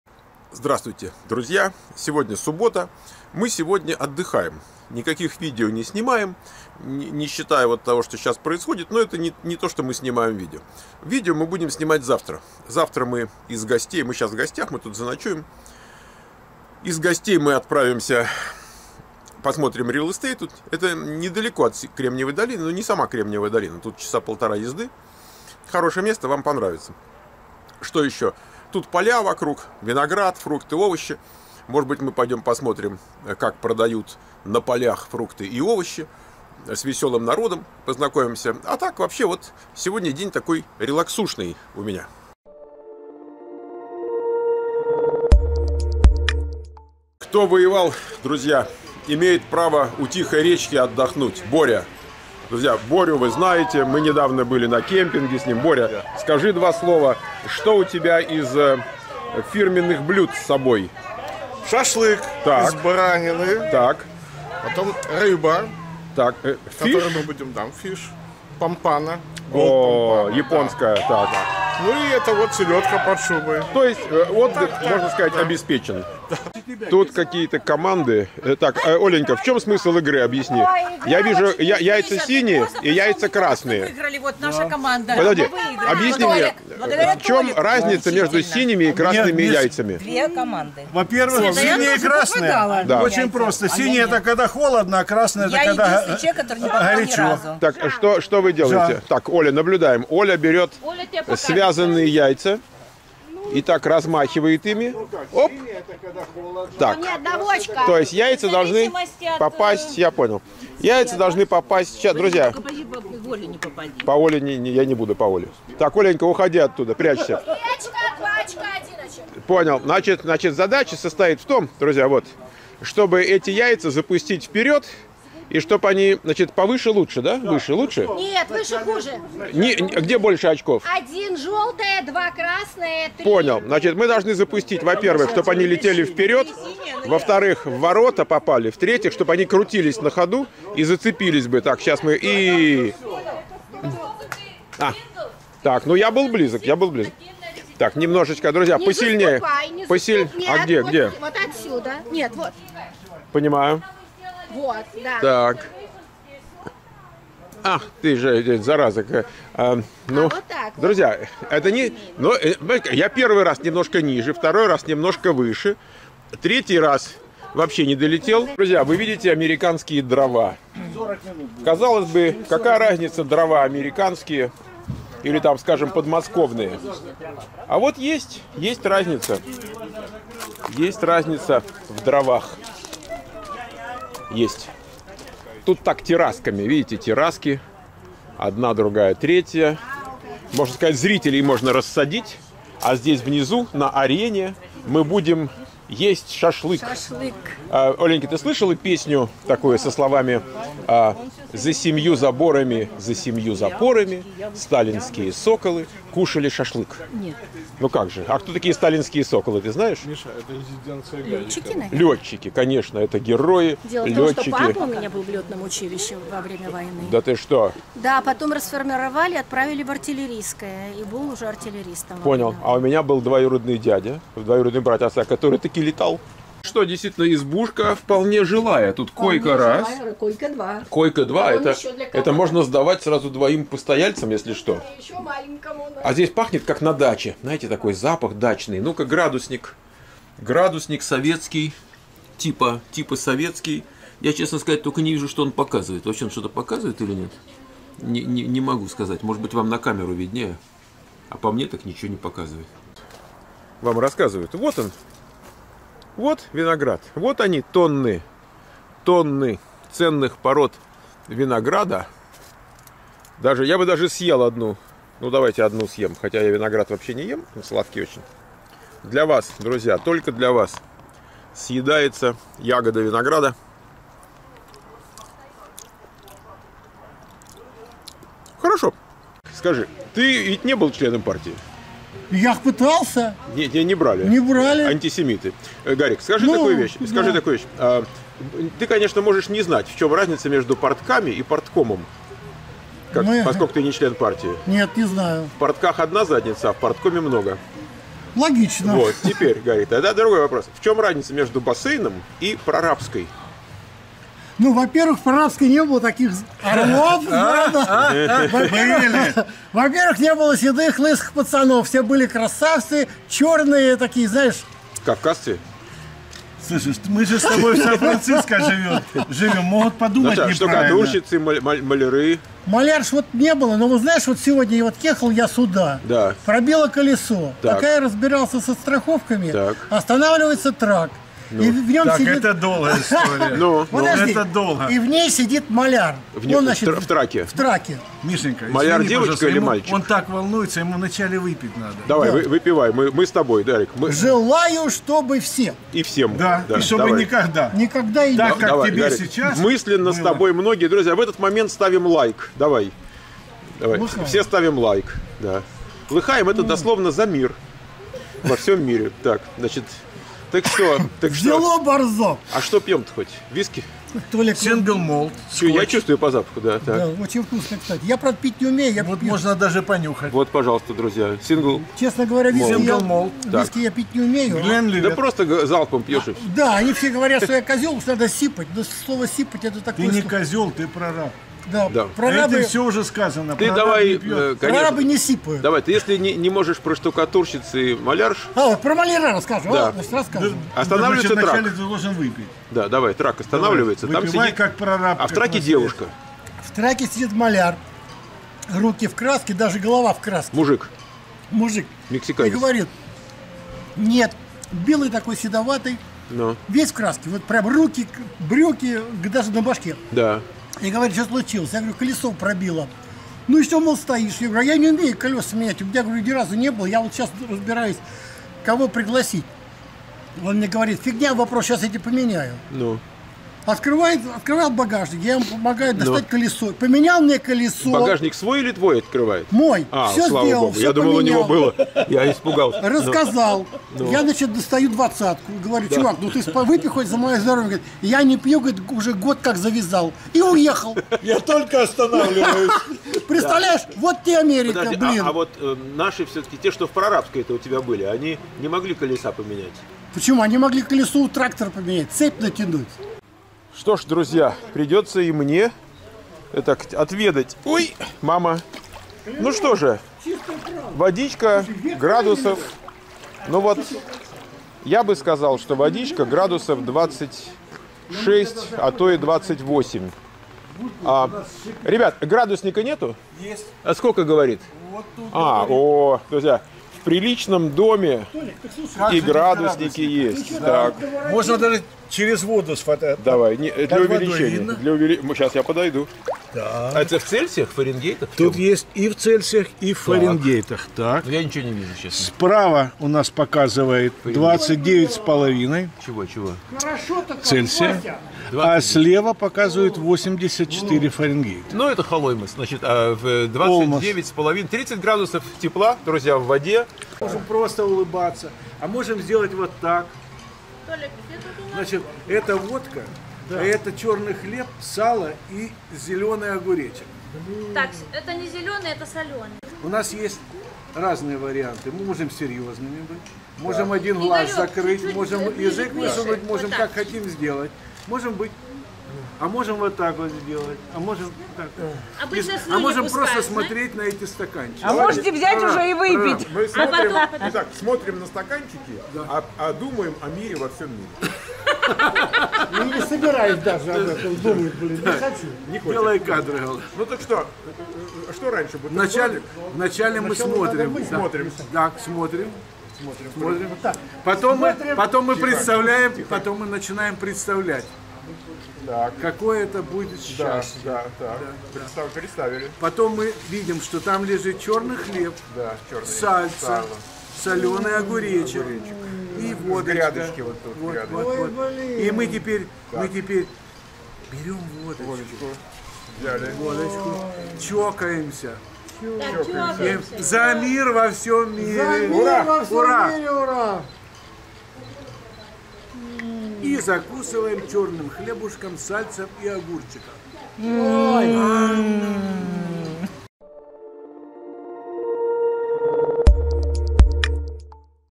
США 5367: суббота - отдыхаем в гостях - 1
Никаких дел, никаких видео - просто тусуемся и расслабляемся
Ссылки на канале SiliconValleyVoice /школа Михаил Портнов:
Самый информативный форум об эмиграции в США: